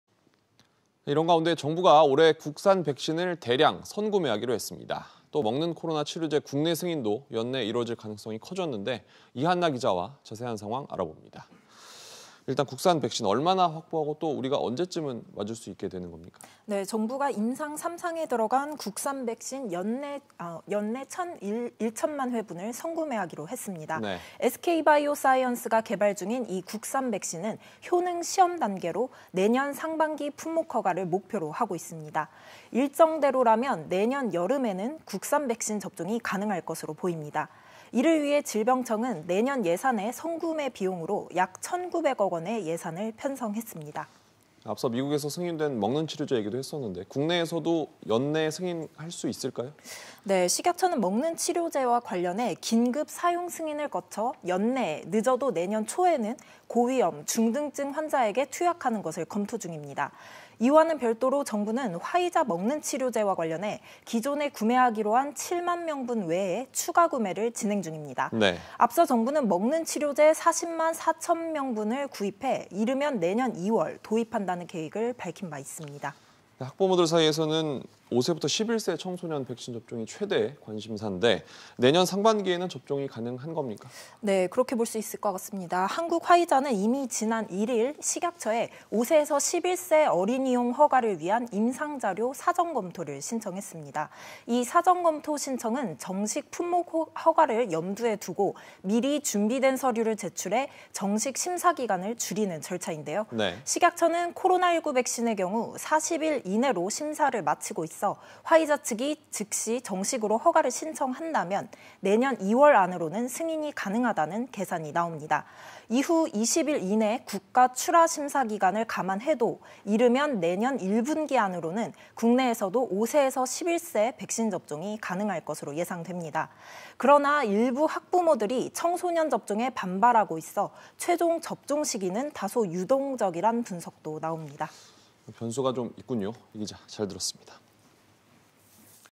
오미크론과 싸울 무기…국산 백신·먹는 치료제 대량구매
[앵커]

국산 백신에 대한 관심이 높은 가운데, 정부가 올해 국산 백신을 대량 선구매하기로 했습니다.

이와는 별도로 먹는 코로나 치료제도 연내 승인키로 가닥을 잡았는데요. 이한나 기자와 이야기 나눠보겠습니다.

이한나 기자, 국산 백신, 얼마나 확보하고 또 우리가 언제 맞을 수 있게 되는 건가요?

[기자]

네, 정부가 임상 3상에 들어간 국산 백신을 연내 1천만 회분을 선구매키로 했습니다.